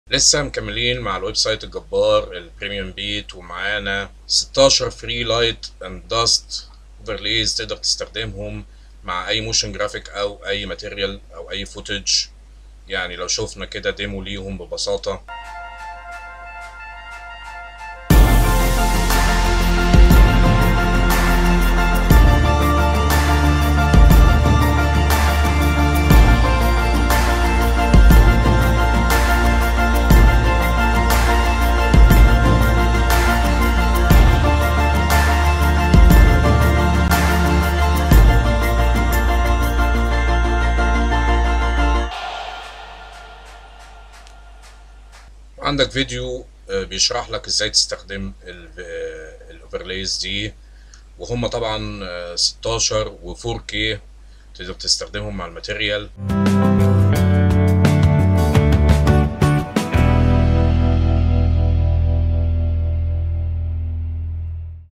16 مؤثر ضوئي و غبار Overlays لل 4K Light and Dust Elements Motion Graphics and Footage
Using 16 FREE Light and Dust Overlays with Motion Graphics and Footage.. 4K Light and Dust Elements .. all overlays are compatible with i After effects,Premiere Pro, DaVinci Resolve, FCPX, and other NLEs.